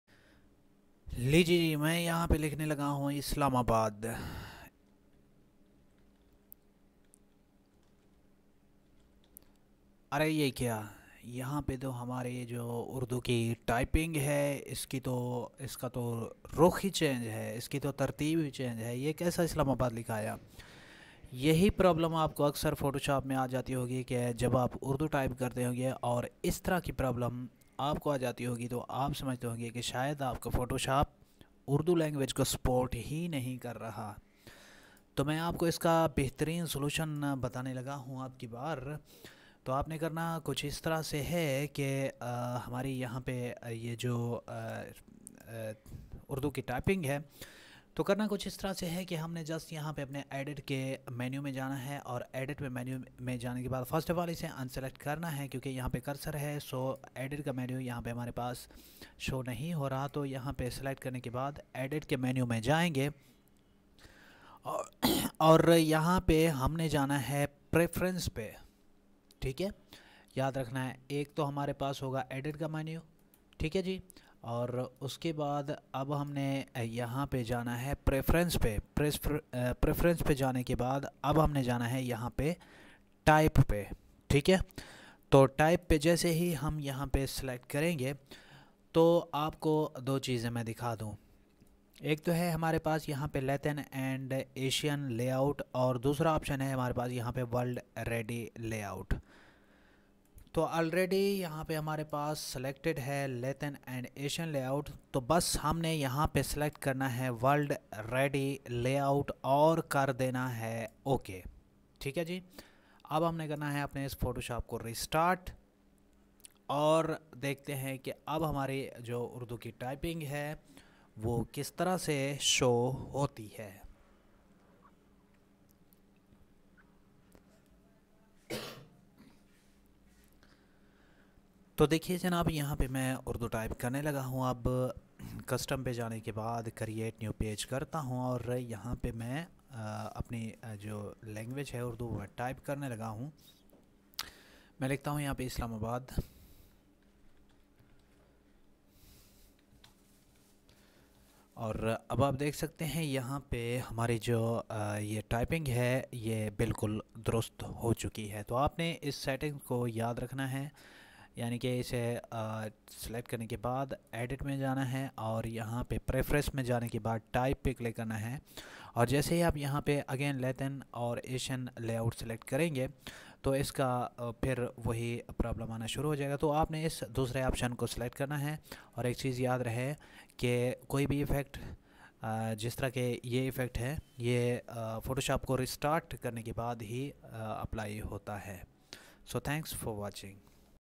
How to write Urdu in Photoshop 2021 | Best trick for photoshop | Gameay
Gameay